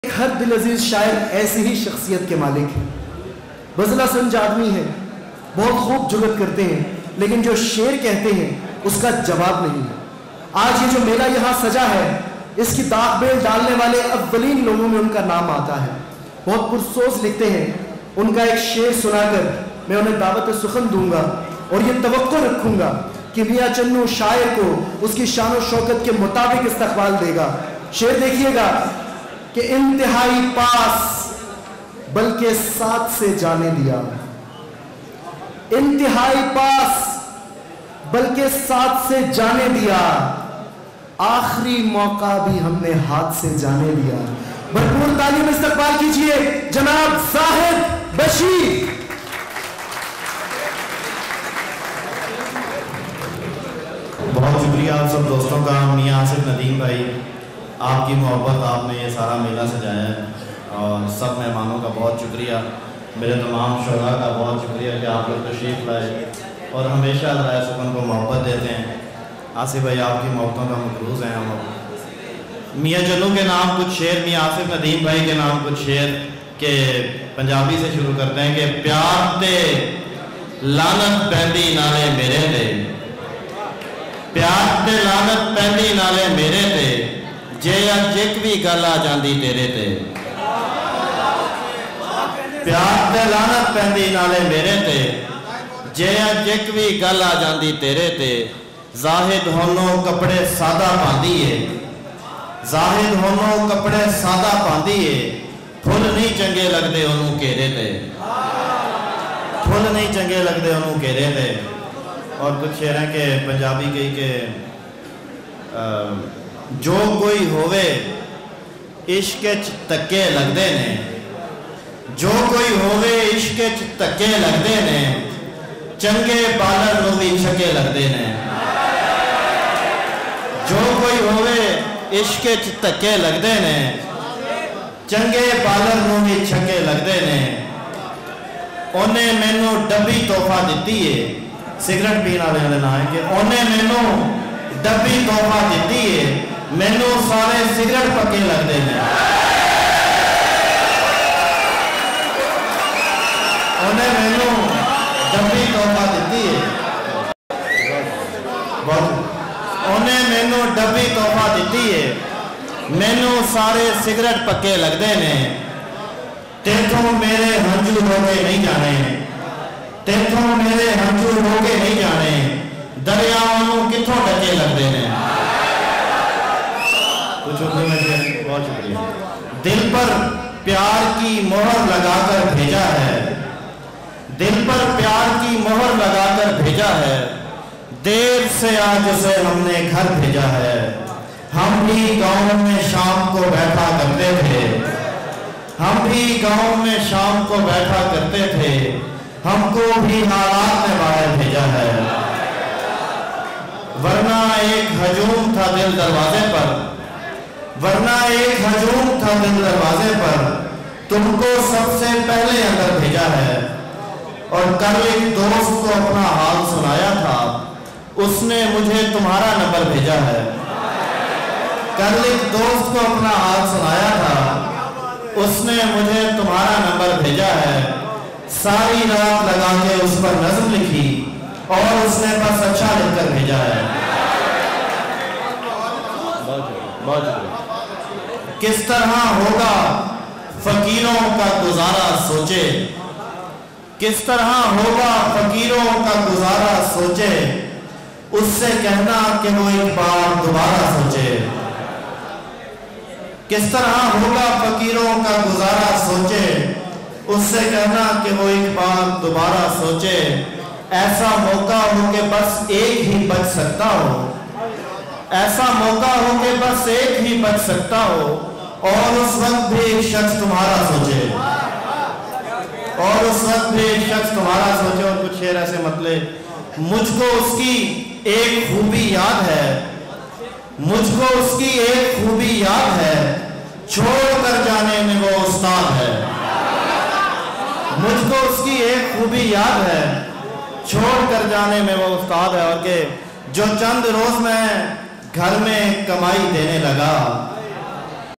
Zahid Bashir | Azrah e Sukhan Mushaira | Urdu Poetry
Watch Zahid Bashir reciting his best poetry in Mushaira held by Team Azrah e Sukhan.

In the present era, we are bringing a series of literary sessions under the platform of "Azrah-e-Sukhan" for the cultural vitality of Urdu and Punjabi poetry.